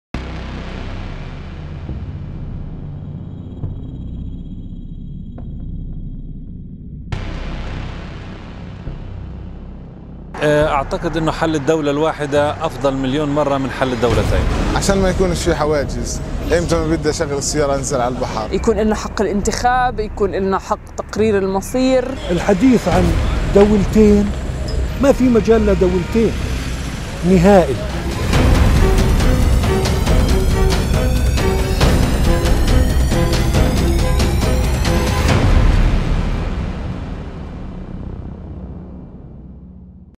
מה הפלסטינים חושבים על תוכנית הימין הקיצוני לספח אותם לישראל?
2.6 מיליון פלסטינים חיים ביהודה ושומרון.
הימין הקיצוני מתכנן לספח אותם לאחר הבחירות.
שאלנו אותם מה דעתם על הסיפוח.

-- סיפוח טוב לפלסטינים --

[40 שנ']